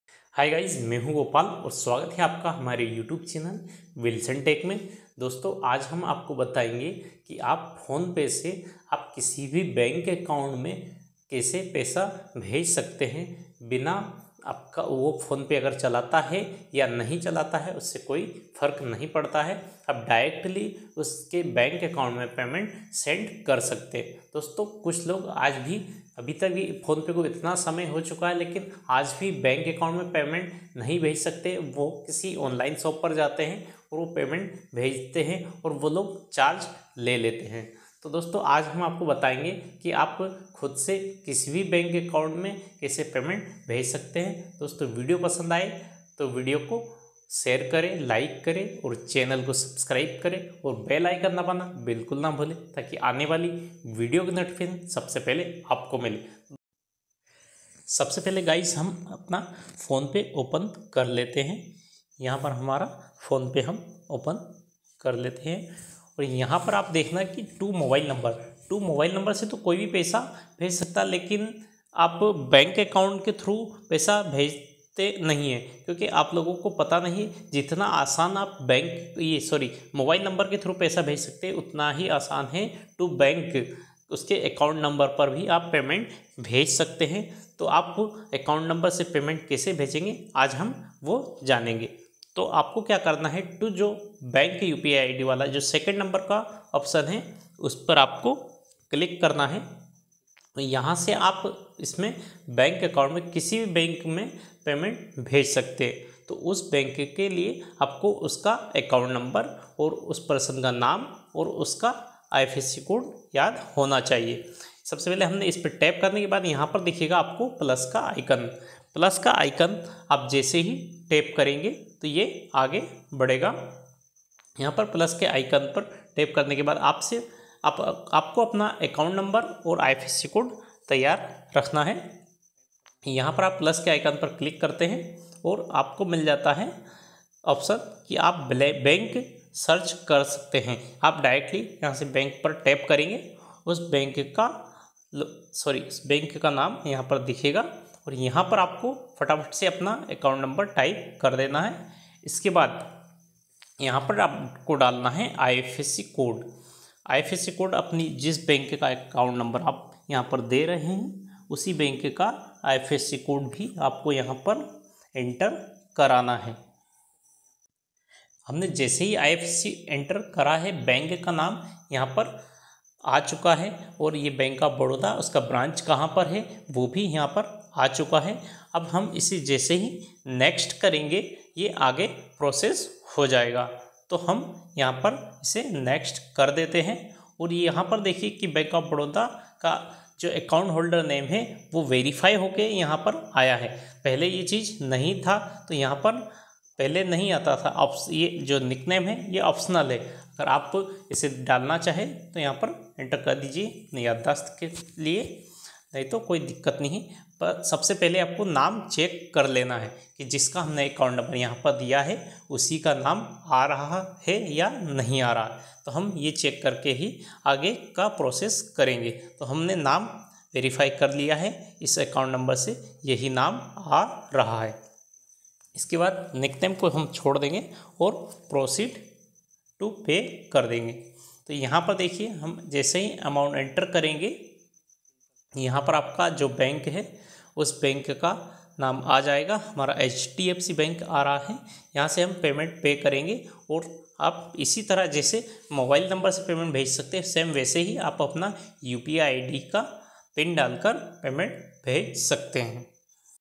Bina mobile number ke phone pe se paisa bhejen || फोन पे से डायरेक्ट बैंक अकाउंट में पैसा भेजें
Bina mobile number ki phone per se paisa bhejen || फोन पे से डायरेक्ट बैंक अकाउंट में पैसा भेजें ||

video mein humne bataya hai ki aap apne phone pe account se kisi bhi bank account mein Paisa kaise bhej sakte hain turant